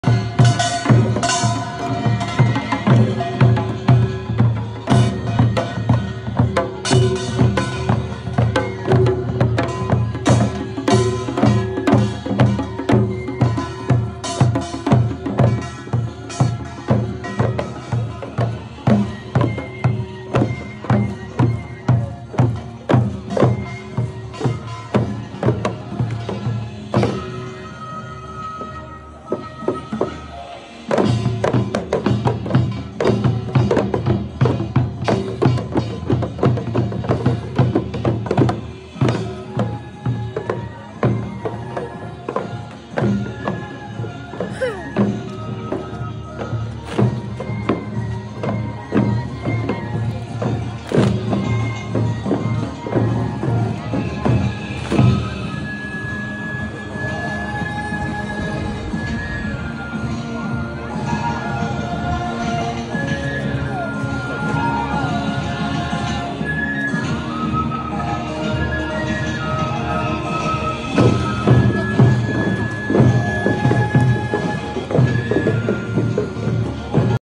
BTS - Korean Folk Village
A quick BTS clip of a show at the Korean Folk Village just outside of Seoul Korea